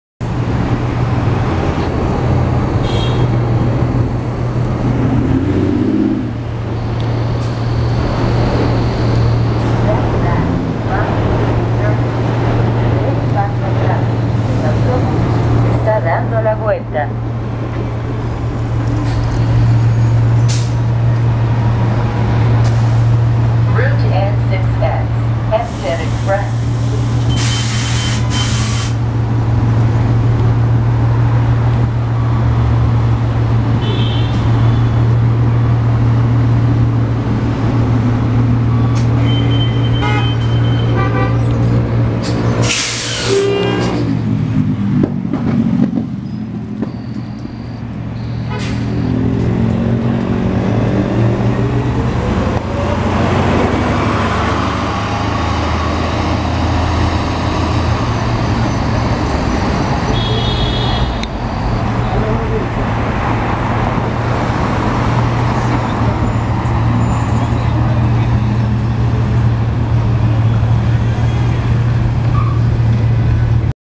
NICE Bus 2017 New Flyer XN60 Xcelsior CNG #1968 On The N6X @ Hillside Avenue & 168th Street
Here is 2017 New Flyer XN60 Xcelsior CNG #1968 on the N6X making it's stop at 168th Street and Hillside Avenue, bound for the Hempstead Transit Center in Hempstead, Nassau County.

Cummins ISL-G & Allison B500R